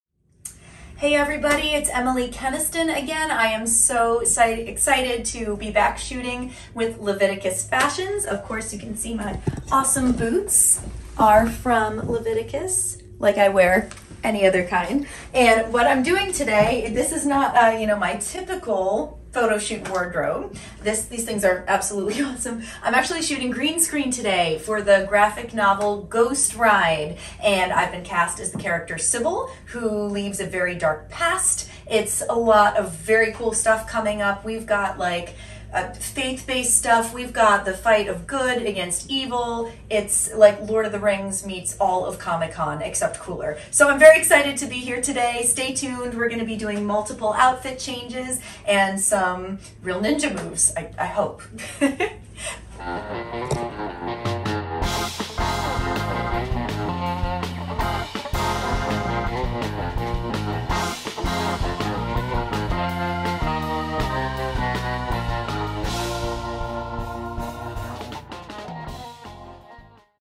ACTRESS MODEL EMILY KENNISTON GREEN SCREEN COSPLAY COMIC BOOK SHOOT BLACK LEATHER THIGH BOOTS 01
Emily Kenniston stopped by to do a full green screen shoot for the new graphic novel / comic book.  Of course her character, Sybil, just had to wear our Leviticus Fashions boot designs as the starring roles in her costumes!